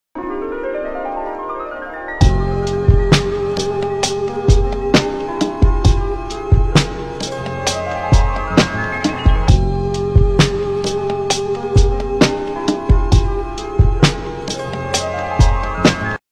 TAKASHI MURAKAMI IS THE DEFINITION OF ART 🎨🤯
Tour of the Takashi Murakami Art Exposition